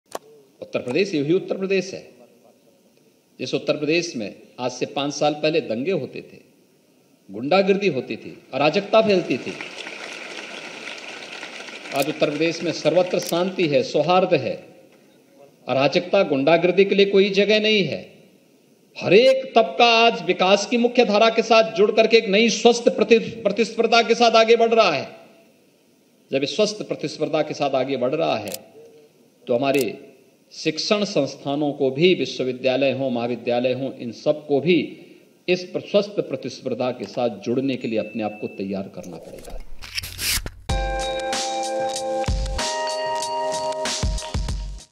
'UP saw multiple riots & violence 5 years back, today there is complete peace': Yogi Adityanath
Uttar Pradesh chief minister Yogi Adityanath today said, 'Uttar Pradesh saw multiple riots & violence 5 years back, today there is complete peace'